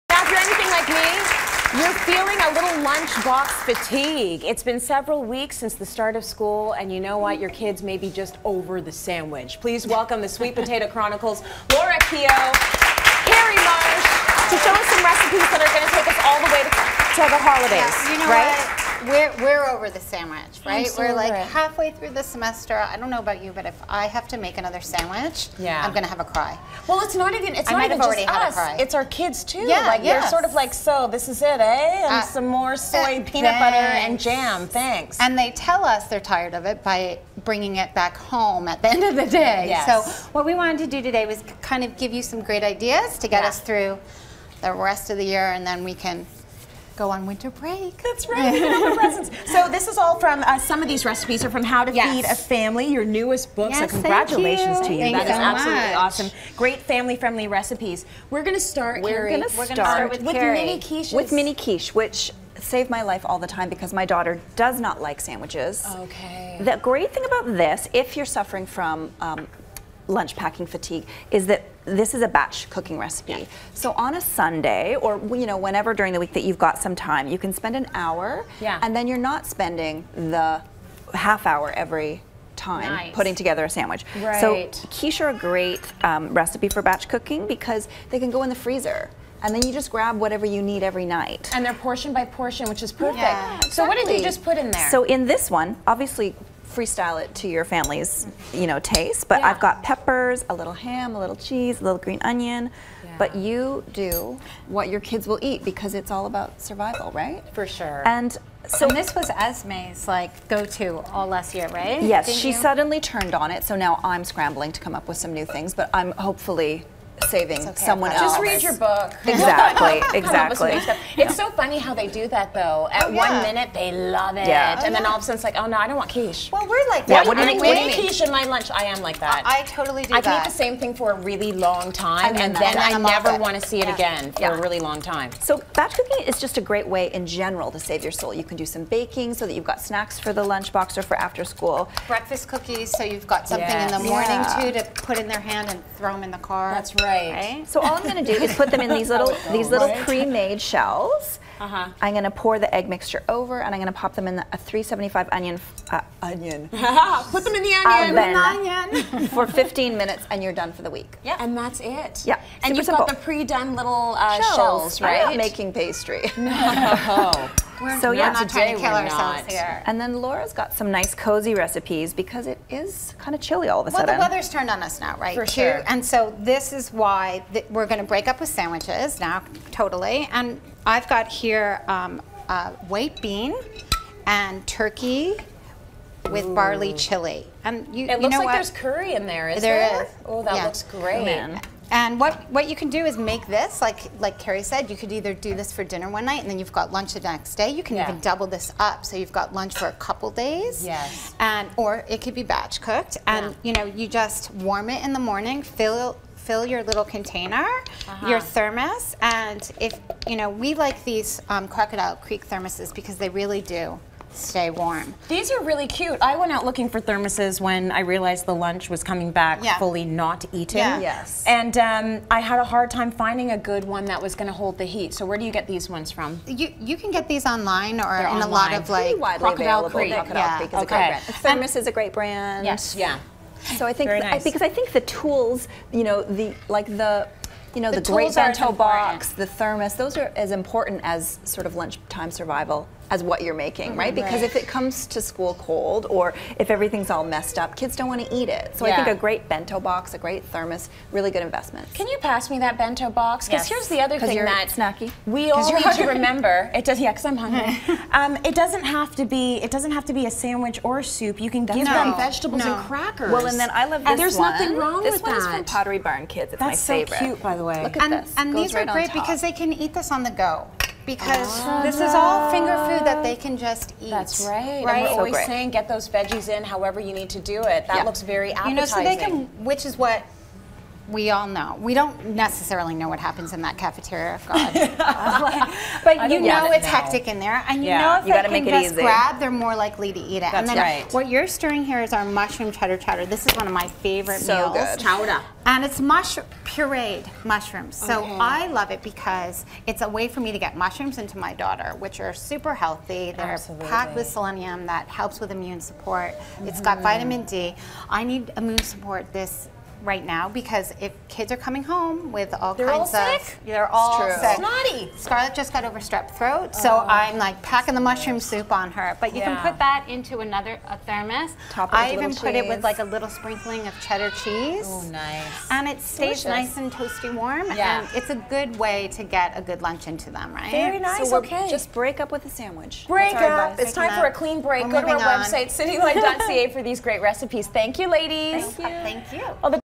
Sandwich alternatives to get out of a school lunch rut
Are you tired of packing sandwiches for lunch? Here are some great alternatives to pack for your kids this week.